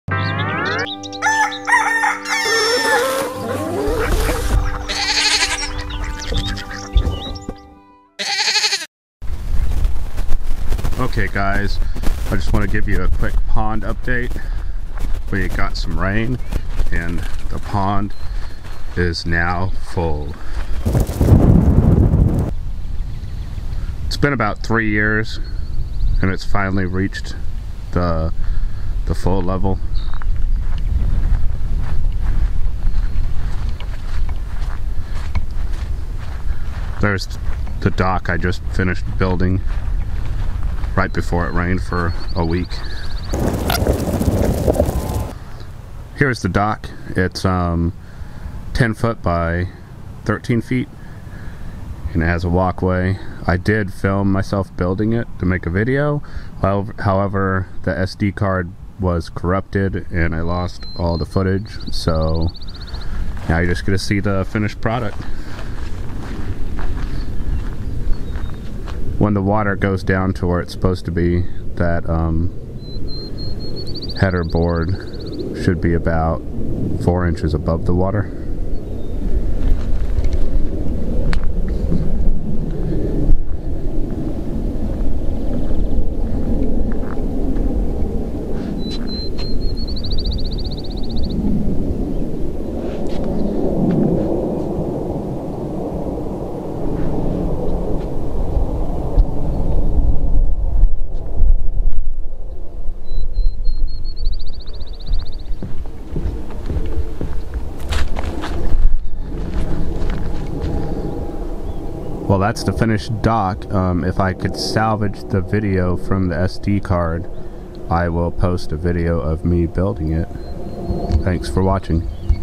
Built a Dock For The Pond
Quick update on the pond because it filled up and the dock is done. I finally finished building the dock. Lost the footage of building the dock. Also made a new intro for the channel.